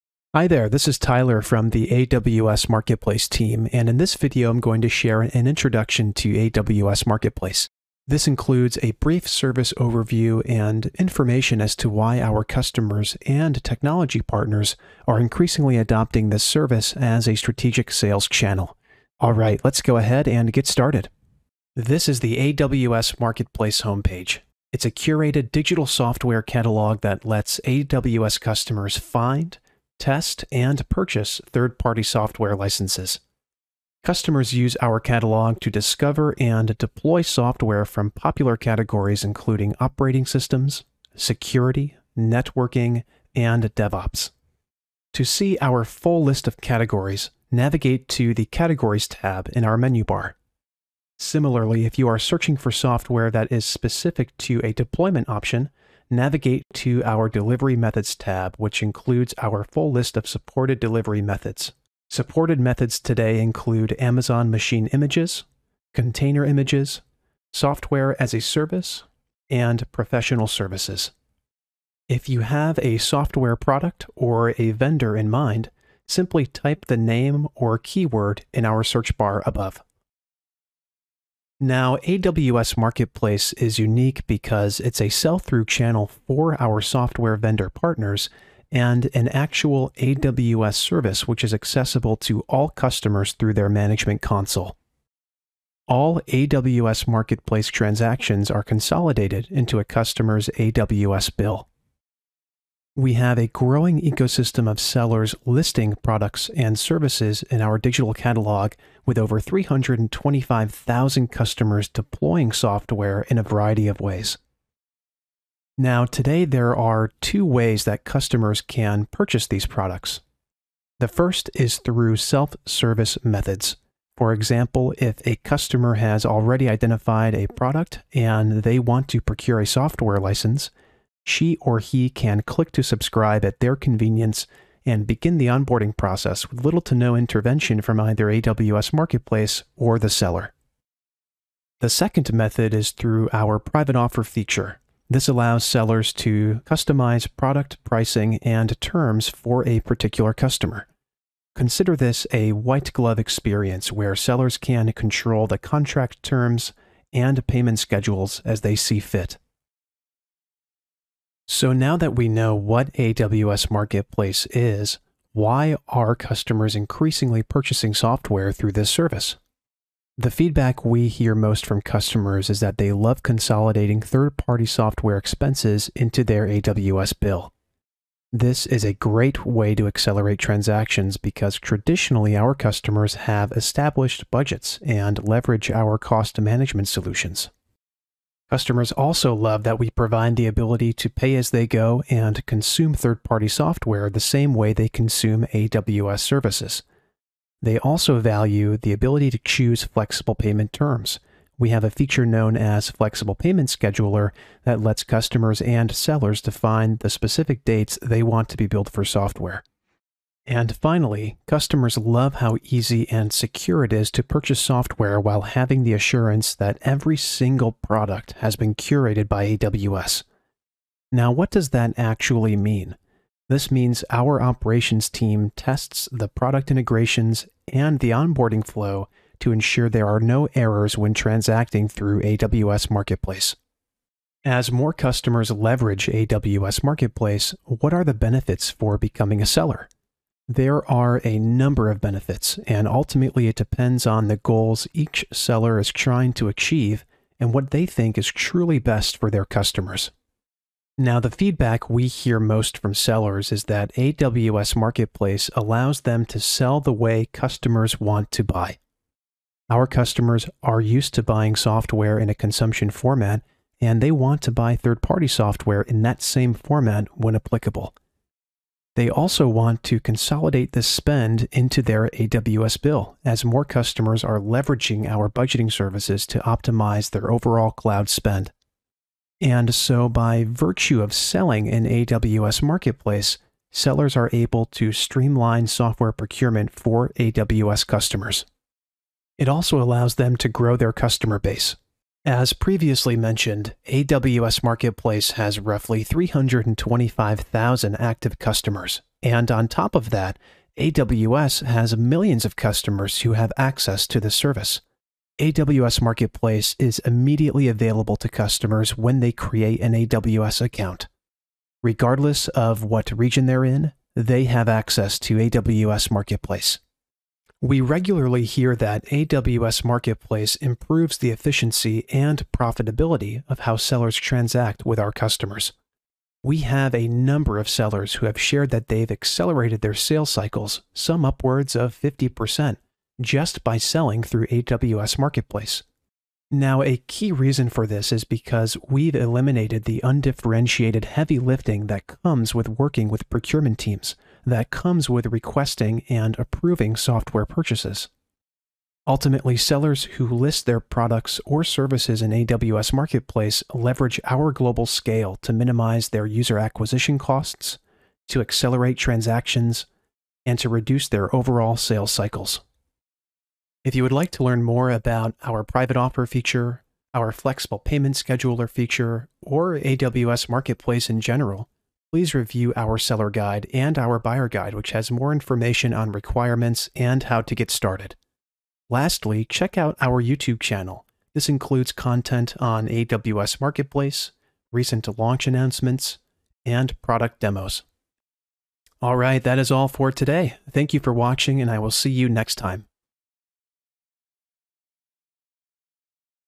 Introduction to AWS Marketplace | Amazon Web Services
AWS Marketplace is a curated digital catalog that makes it easy for you to find, buy, deploy, and manage third-party software licenses. Our catalog includes thousands of software listings in popular categories such as networking, security, storage, machine learning, and more.

To learn more, please review our seller guide which includes more information on seller requirements and how to get started: